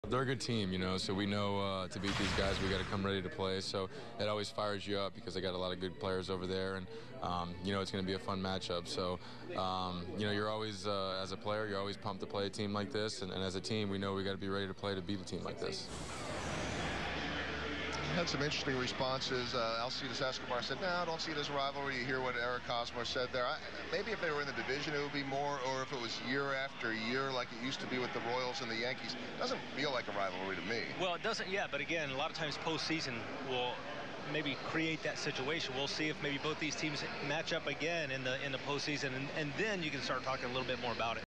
Is Royals-Angels a rivalry?
Are the Royals and Angels developing a real rivalry? Eric Hosmer, Joel Goldberg and Jeff Montgomery weigh in.